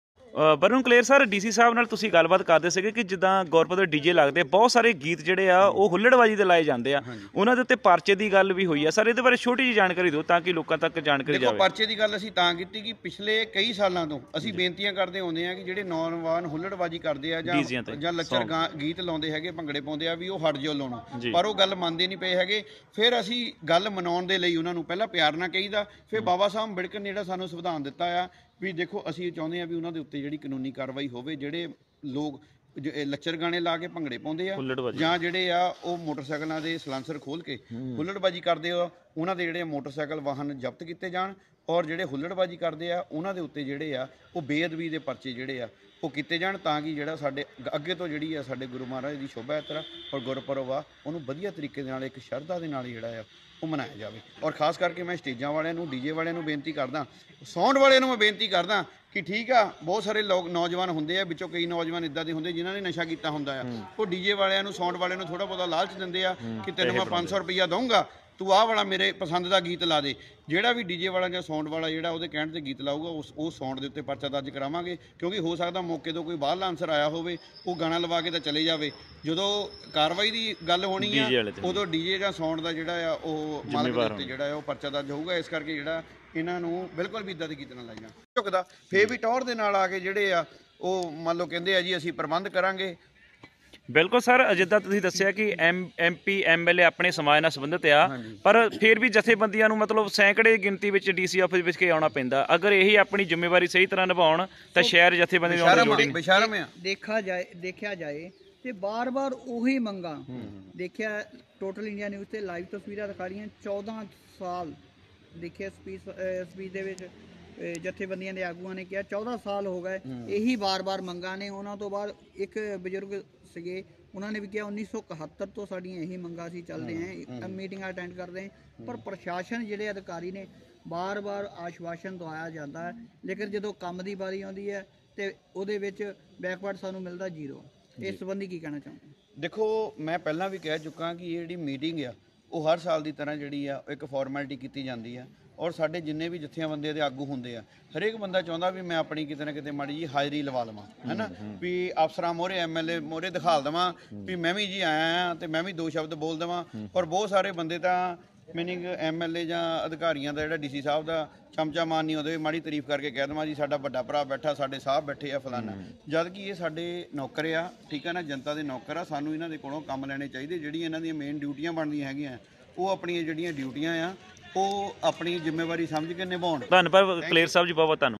Jalandhar Shobha Yatra Gurpurab ਦੌਰਾਨ ਹੁਲ_ੜ_ਬਾਜ਼ੀ ਕਰਨ ਵਾਲੇ ਨੋਜਵਾਨਾਂ ਹੋ ਜਾਣ ਸਾ_ਵਧਾਨ: Varun Klair
Jalandhar Shobha Yatra Gurpurab ਦੌਰਾਨ ਹੁਲੜਬਾਜ਼ੀ ਕਰਨ ਵਾਲੇ ਨੋਜਵਾਨਾਂ ਹੋ ਜਾਣ ਸਾਵਧਾਨ: Varun Klair

Shri Guru Ravidass Maharaj Ji ਦੇ ਗੁਰਪੁਰਬ ਅਤੇ ਸ਼ੋਭਾ ਯਾਤਰਾ ਦੌਰਾਨ ਹੁਲੜਬਾਜ਼ੀ ਕਰਨ ਵਾਲੇ ਨੋਜਵਾਨਾਂ ਤੇ ਹੋਵੇਗੀ ਕਾਨੂੰਨੀ ਕਾਰਵਾਈ
ਧਾਰਮਿਕ ਪ੍ਰੋਗਰਾਮ ਦੌਰਾਨ ਧਾਰਮਿਕ ਗੀਤ ਹੀ ਲਗਾਏ ਜਾਣ : ਵਰੁਣ ਕਲੇਰ ਬੂਟਾਂ ਮੰਡੀ

Dist. frojpur Atari DrJatinderSinghMattu dalit bycard breakingnews 22 LIVE News 22 ਲਾਈਵ ਨਿਊਜ